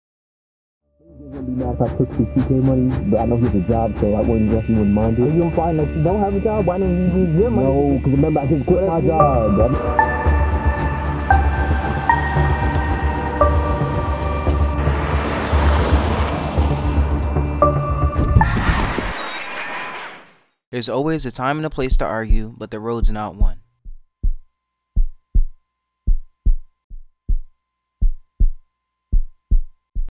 Cheyanne - DON'T ARGUE WHILE DRIVING PSA
Cheyanne Johnson produced this PSA for the Middlesex County "Don't Drive Distracted" Contest.as a  TV class project.